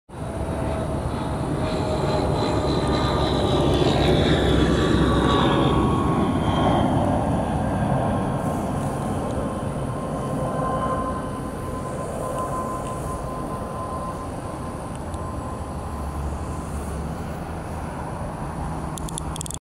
개불알풀꽃에 날아든 등애
개불알풀꽃에 날아든 등애  HorseFly
봄입니다. 사무실 근방 산책중에 양지바른 풀밭 등에  개불알풀꽃이 한창 피어있네요.
손톱보다 작은 풀꽃이 오밀조밀 많이 도 피어있었는데  그사이 사이 등애가 날아다니네요.